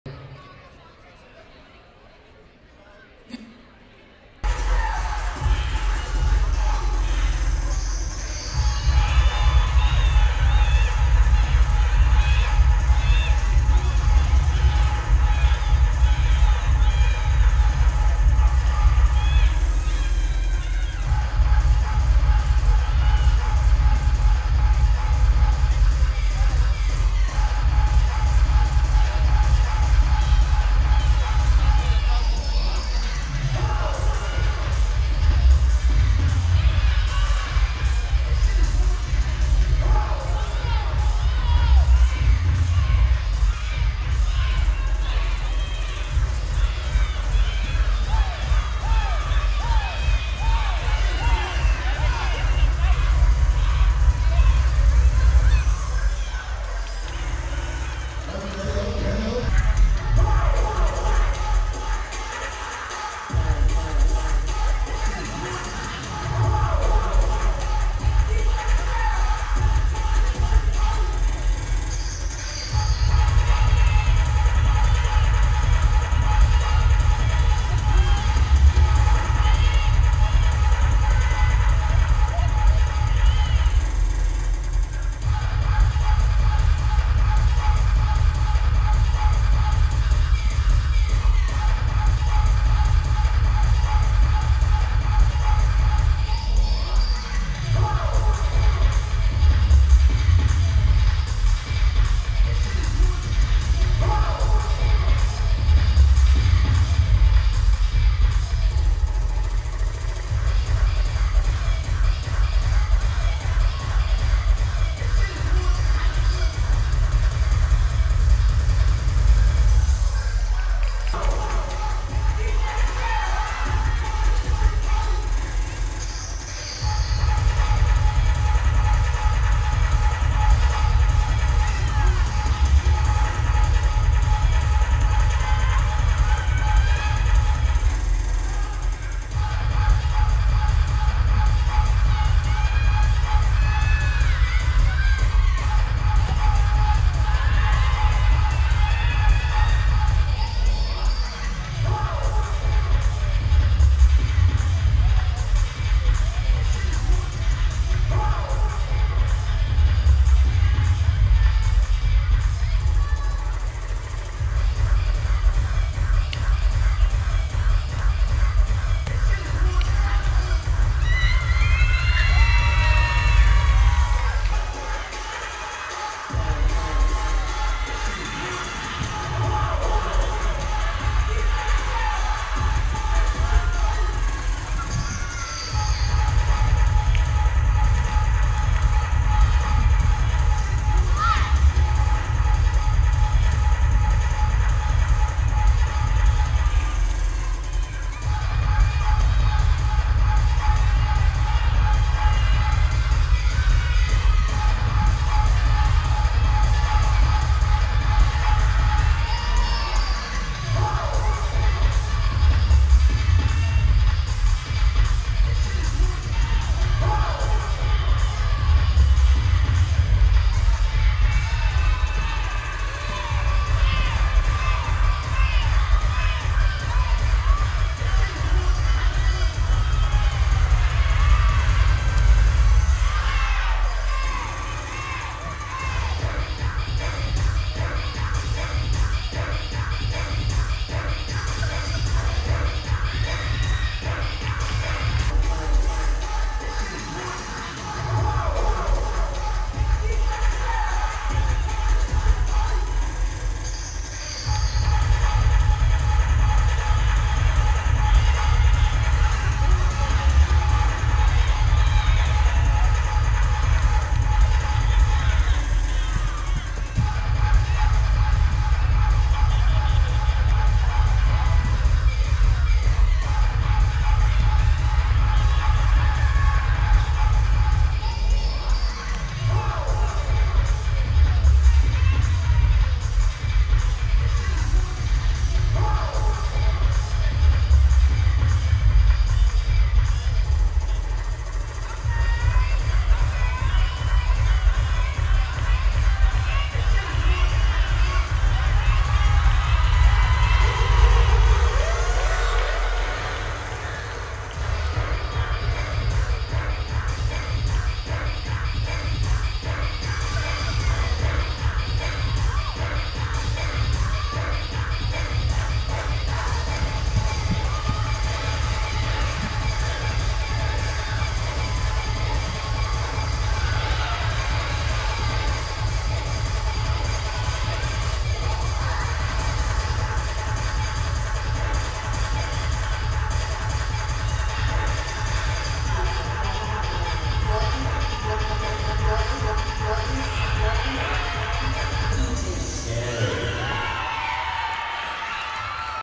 Shimmering Divas 5th Annual Boo or Buck Competition | Division 1 Minis Stand Battle | Round 1
1. Royalty Fierce Elite
2. Dynamic Elite Dance
3. Royalty Elite Dynasty
4. Evolution Dance Academy
5. Sassy Pantherettes

5th Annual Boo or Buck Competition
Hosted by Shimmering Divas Dance Team in Forth Worth, Texas